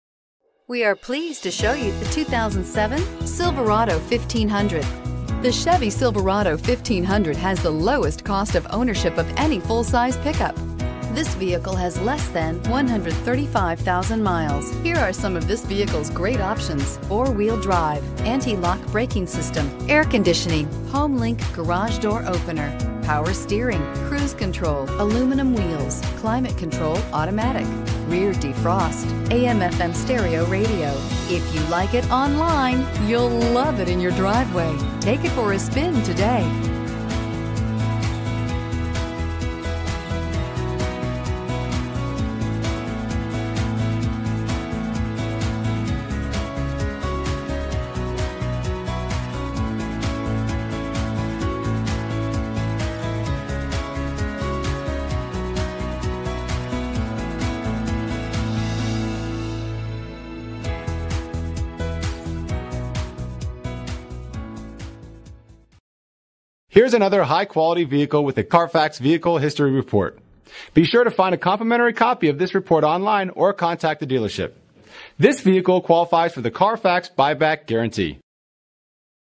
2007 Chevrolet Silverado 1500 Lawrenceville GA K0273
2007 Chevrolet Silverado 1500 LT w/1LT

2007 Chevrolet Silverado 1500 LT Vortec 5.3L V8 SFI Flex Fuel 4-Speed Automatic with Overdrive 4WDbrbrSilverado 1500 LT, Vortec 5.3L V8 SFI Flex Fuel, 4-Speed Automatic with Overdrive, 4WD. Please don't hesitate to call Lauren Whitcraft @ for all information or email 24/7 . Talk to you soon!!brbrbrAwards:br * NACTOY North American Truck of the Year